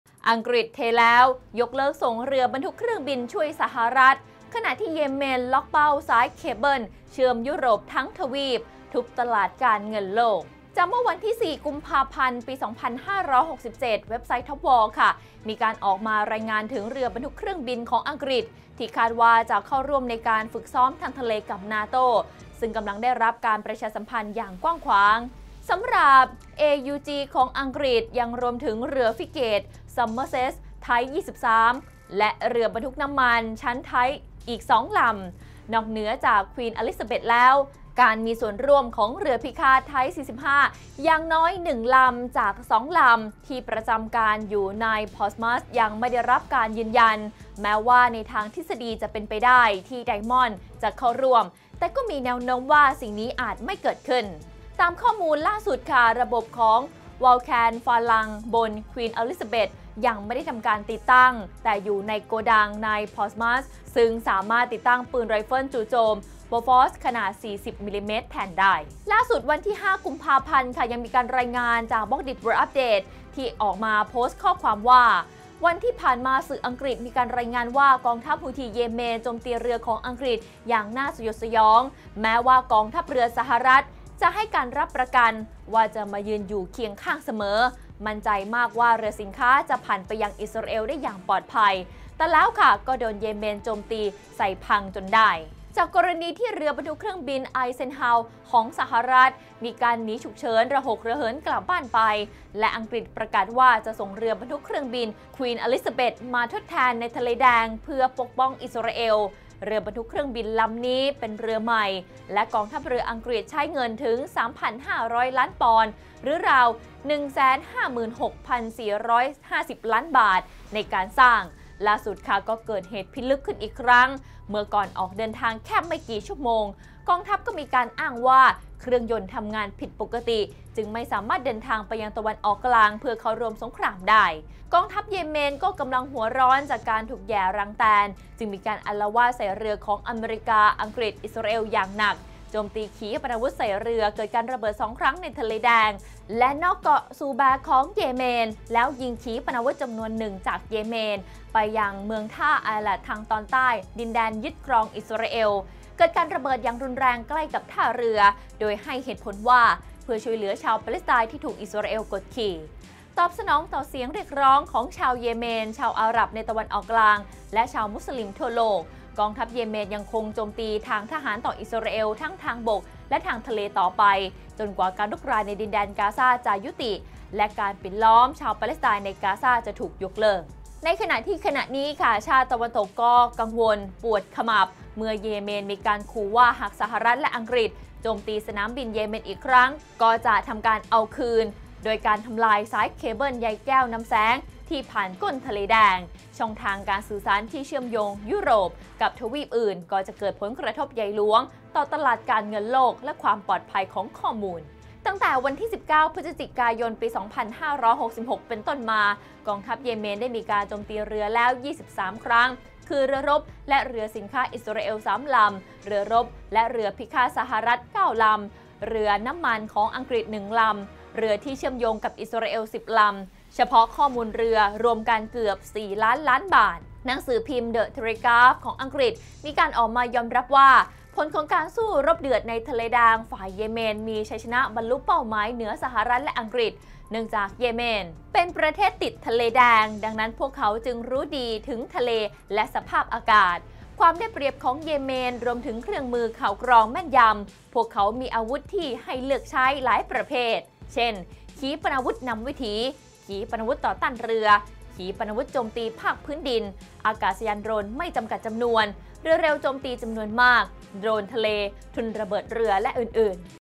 อังกฤษยกเลิกส่งเรือบรรทุกเครื่องบินช่วยสหรัฐ! เยเมนล็อคเป้าสายเคเบิลฯเชื่อมยุโรปทั้งทวีป
อังกฤษยกเลิกส่งเรือบรรทุกเครื่องบินเยเมนล็อคเป้าสายเคเบิลฯเชื่อมยุโรป ทุบตลาดการเงินโลก